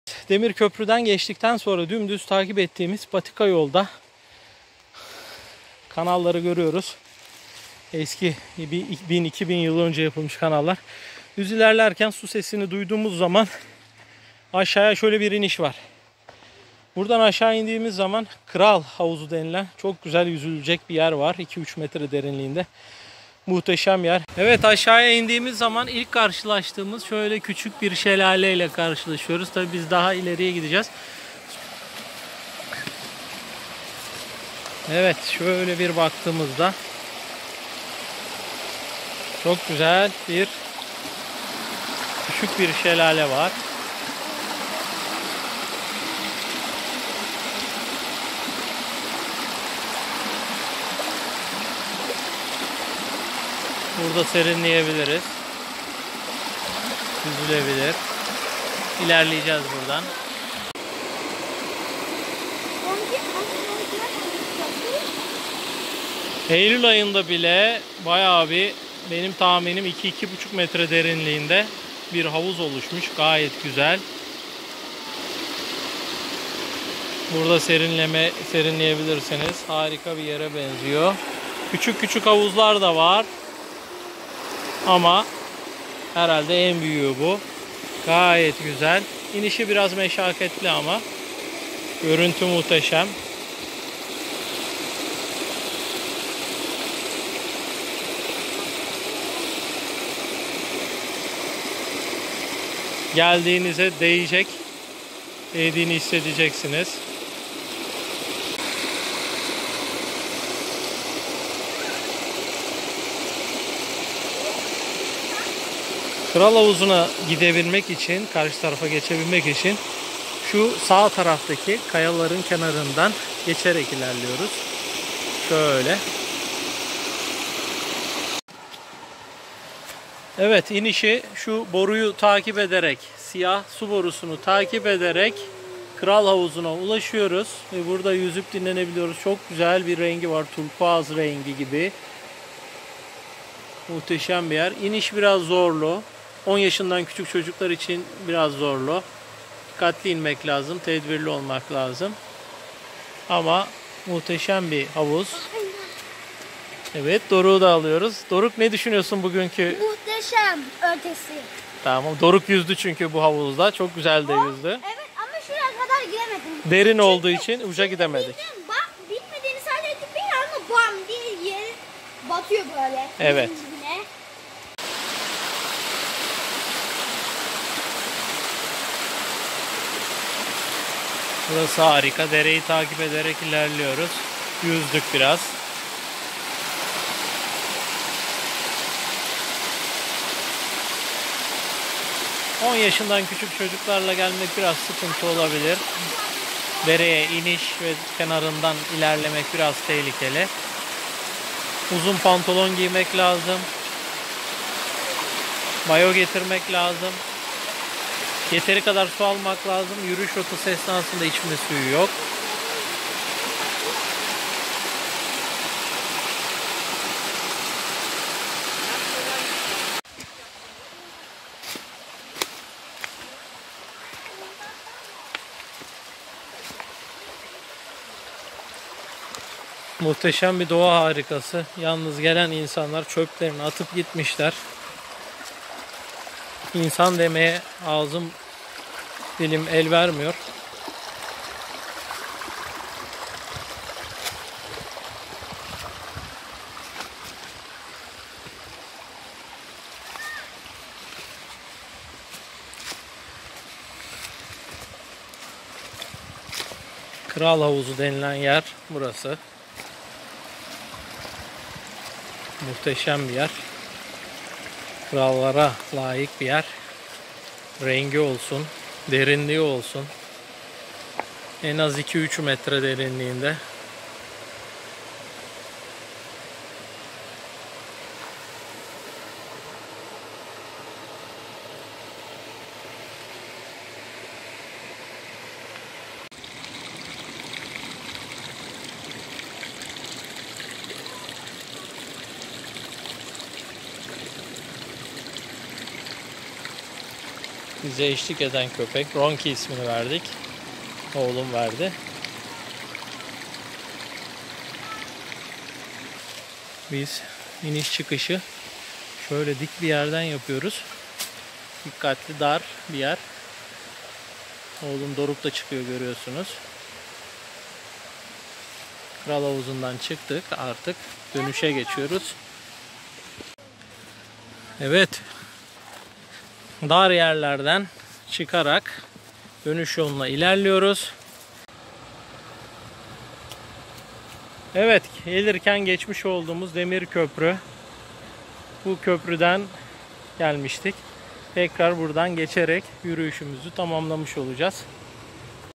Paşasuyu Kral havuzu
Paşasuyu Kral havuzu, Kocaeli'nin İzmit ilçesinde bulunan Hocaoğlu köyünden gidiliyor.
Paşa Suyu suyolu tipik bir Roma suyolu kalıntısıdır. M.S. II. yy başında (111-113) Nikomedeia'dan Bthynia Eyaletinin Valilik görevini yürüten Plınıus (Gaius Plınıus Luciifilius Caecilius Secundus ) tarafından yaptırılan ve Roma Su mühendisliği, mimarlığı harikasıdır. Roma dönemine ait yapılan ikinci büyük su kanalıdır.